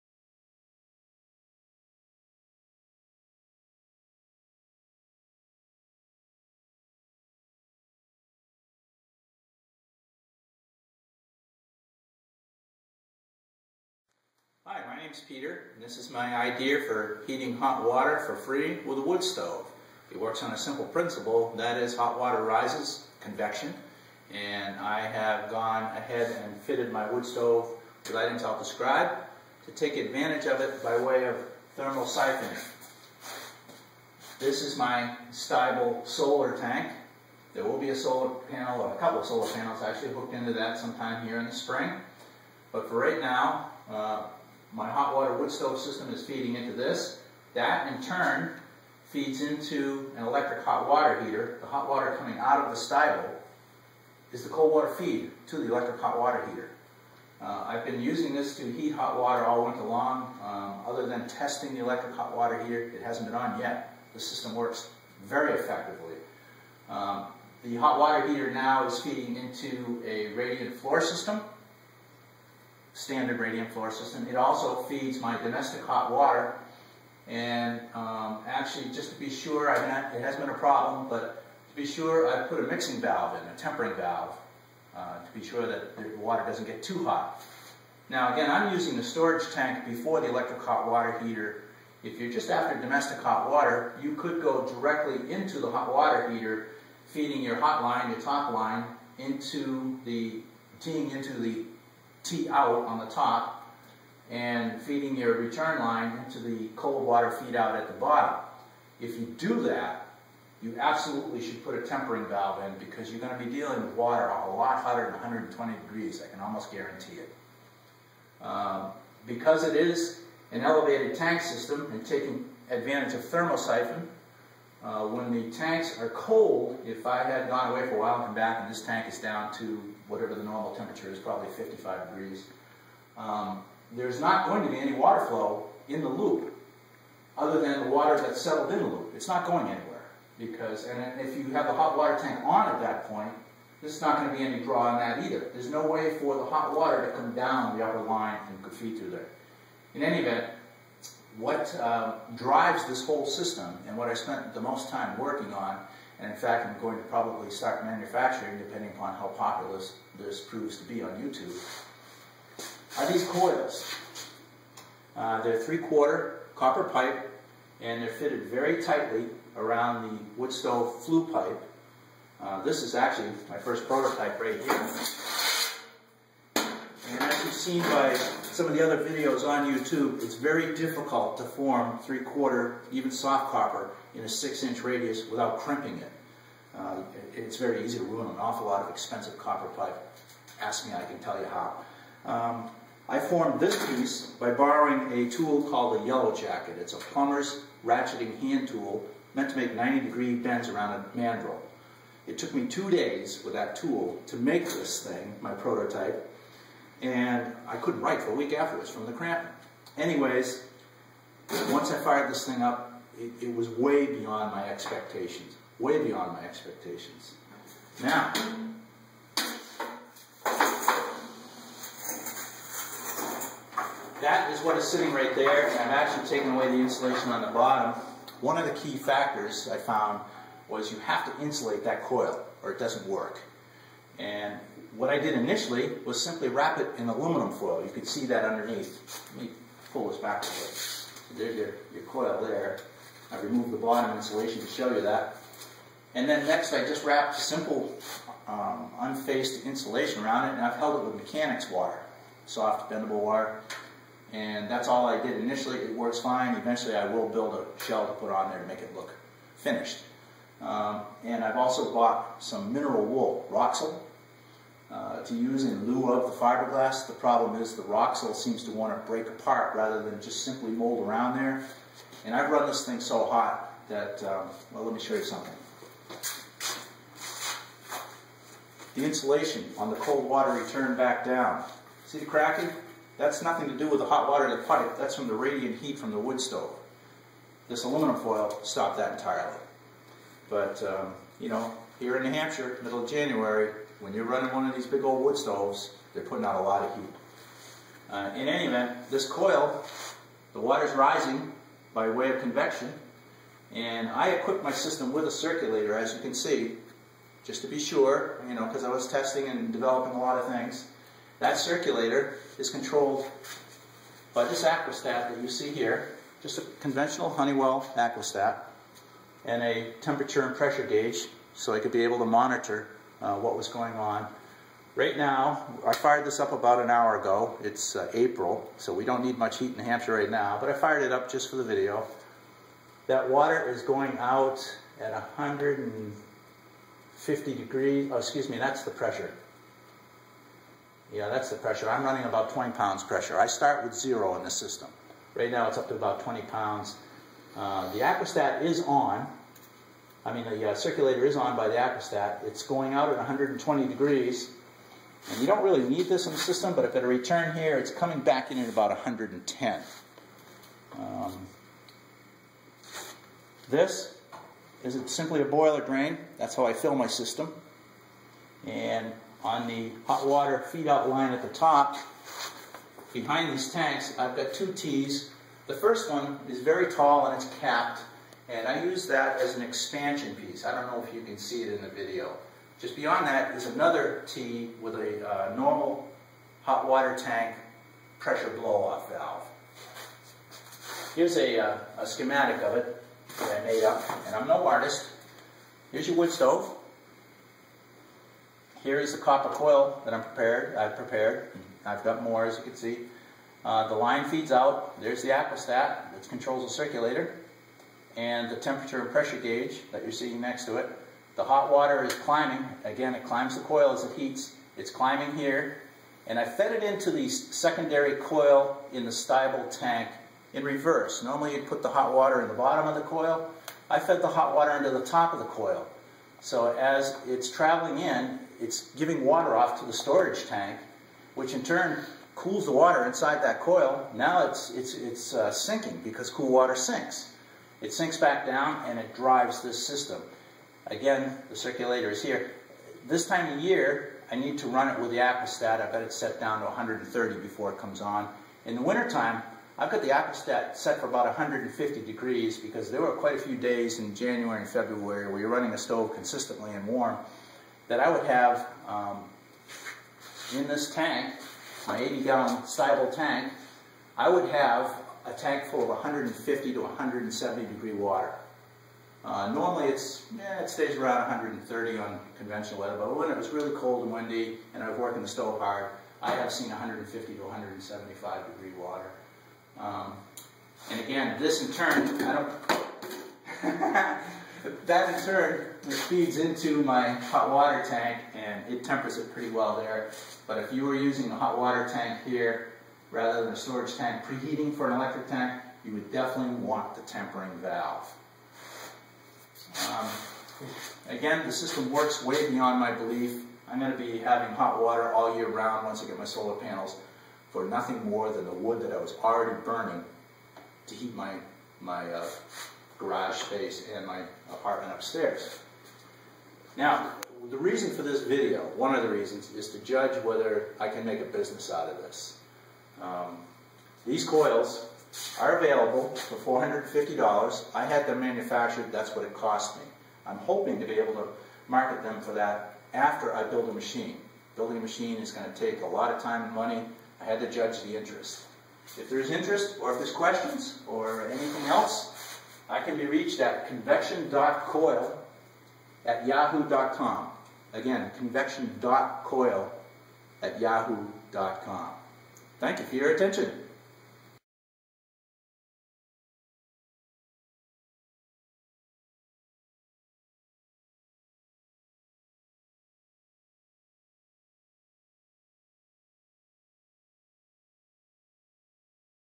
How to get free hot water from your woodstove
A simple, effective, and most importantly SAFE way to heat your hot water with a woodstove.  The natural laws of convection and thermosyphon do all of the work.
I would ask that all interested in this concept to take the time to read all of the great comments below before contributing, there is much to be shared and learned!
Sorry, the website/business has been temporarily suspended in order to start a new pv solar business,